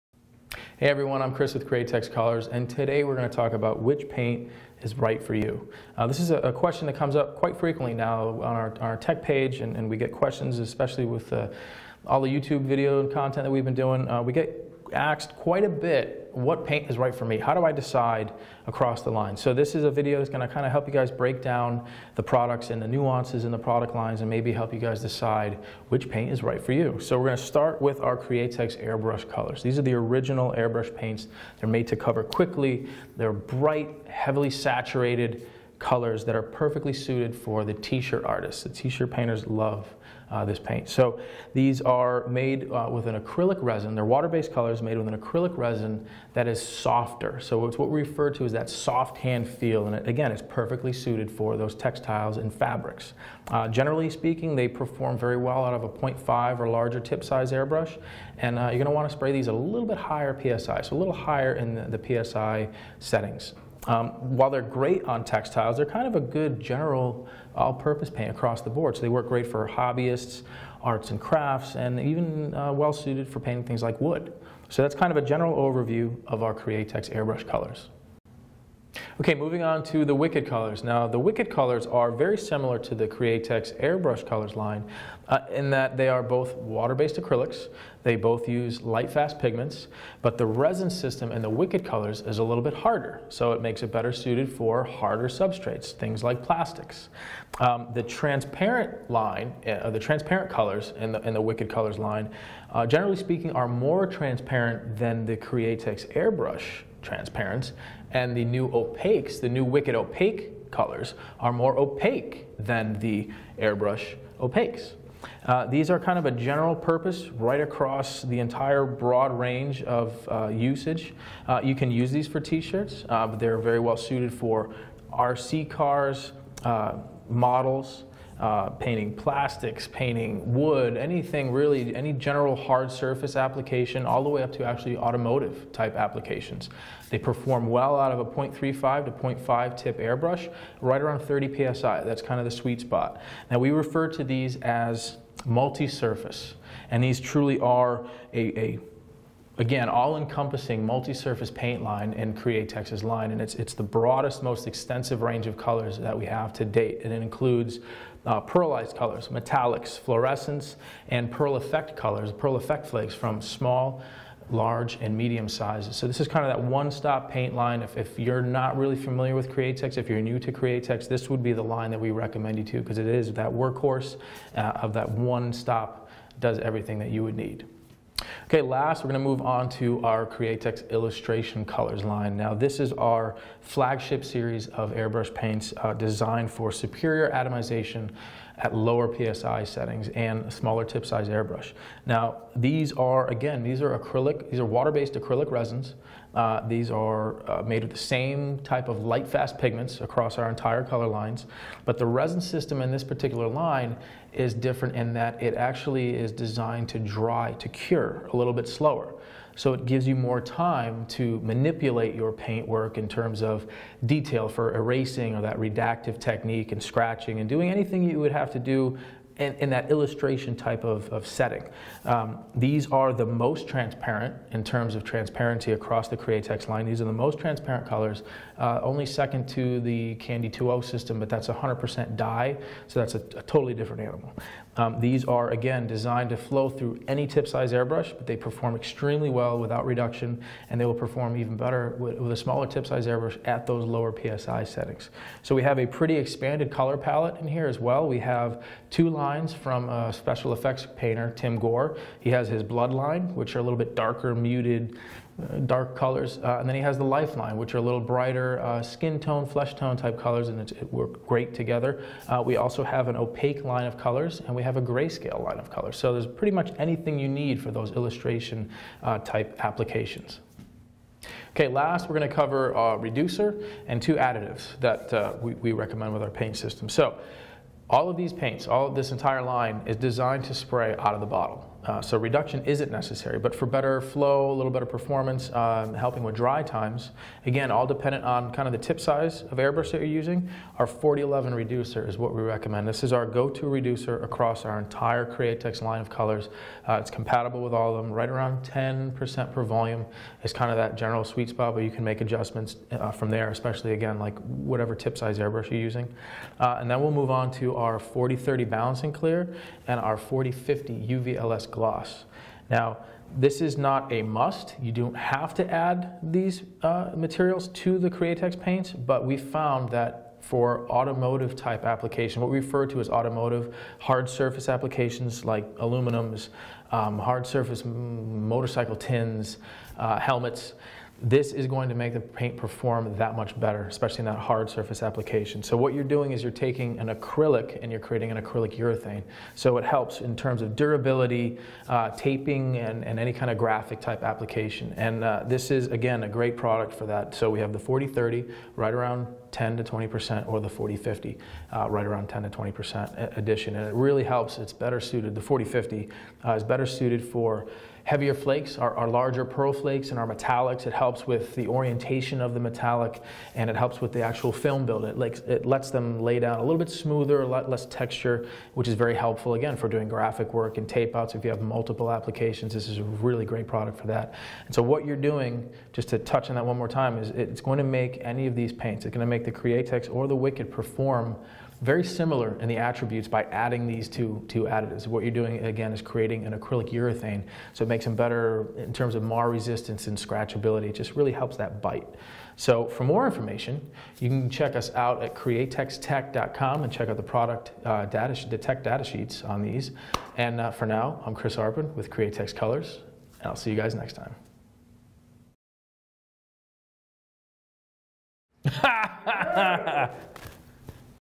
Which Createx Colors Airbrush Paint Do I Need
This video (Which Createx Colors Airbrush Paint Do I Need ) was produced by Createx Colors , showcasing custom painting techniques with Createx Branded water-based paints.